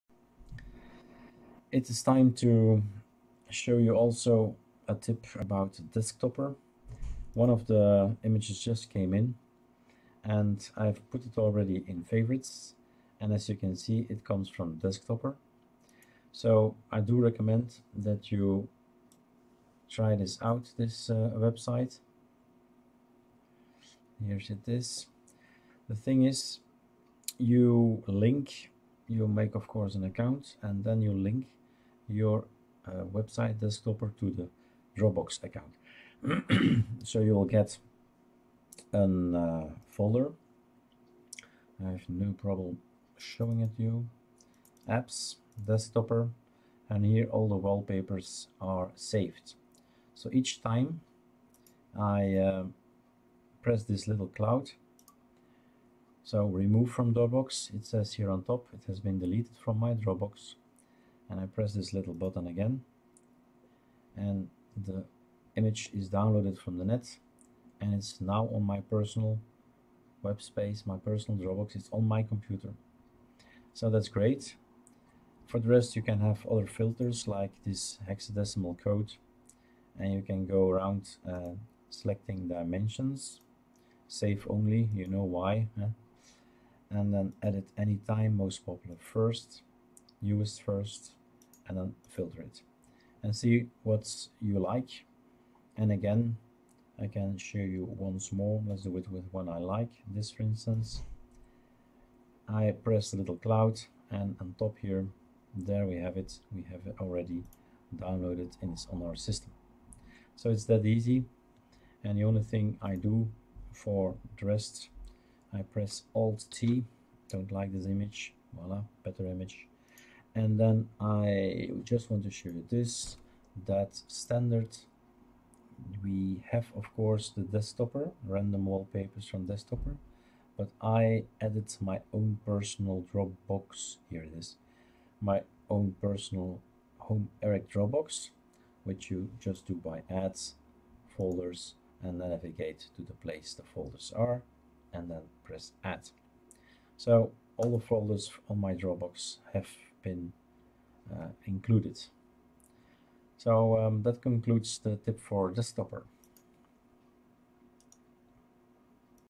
Variety explained 11 : desktoppr and variety go very well together
Variety uses desktoppr as source for the wallpapers. Make an account, link it to your dropbox account and include the dropbox folder from desktoppr as source.